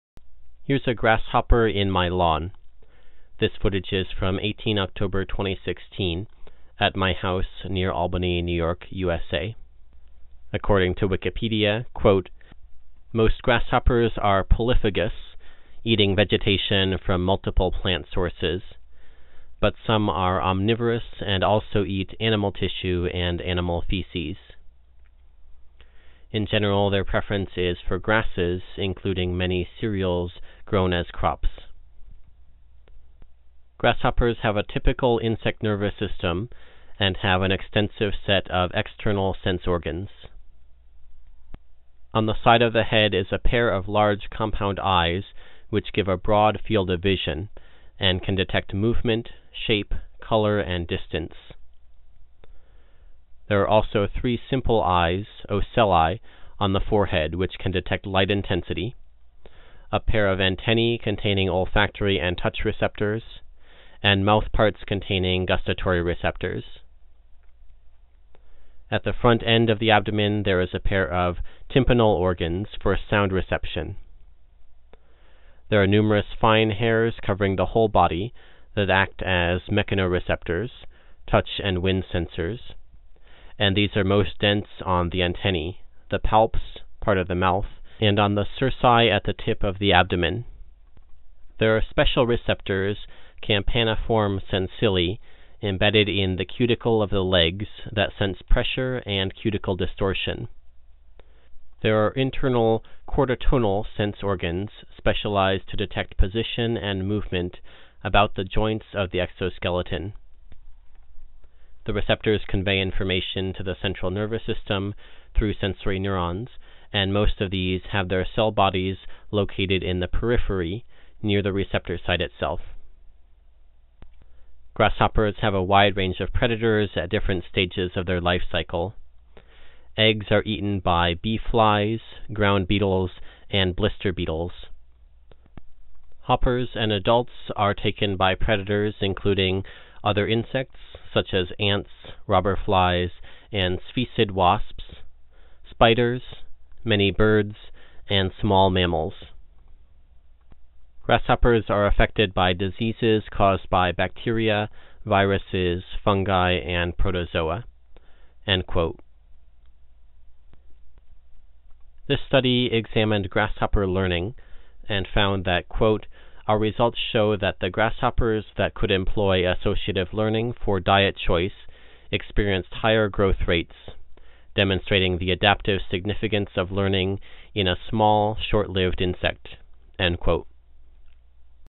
A Grasshopper
This video shows a grasshopper in my lawn and quotes some general information about grasshoppers.